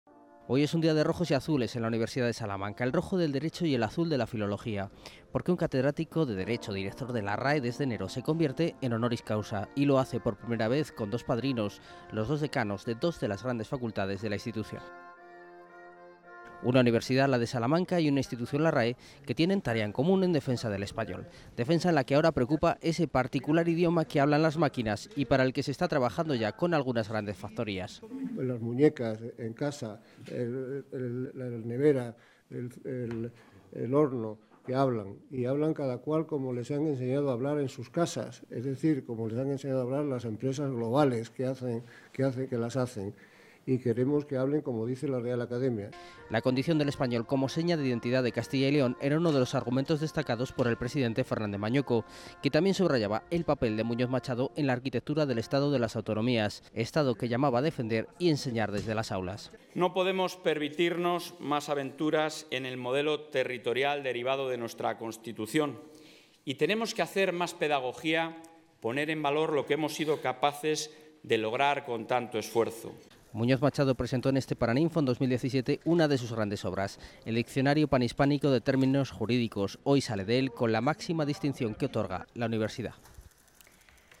Santiago Muñoz Machado, director de la RAE, nuevo doctor Honoris Causa de la USAL
El jurista y director de la Real Academia Española (RAE), Santiago Muñoz Machado, apostó porque las máquinas que marcan y rodean en el día a día a todas las personas, "hablen como dice la RAE" y no como así programan las grandes empresas globales, como Google. Además, aseguró que "el español no sufrirá daños ante la llegada de los anglicismos".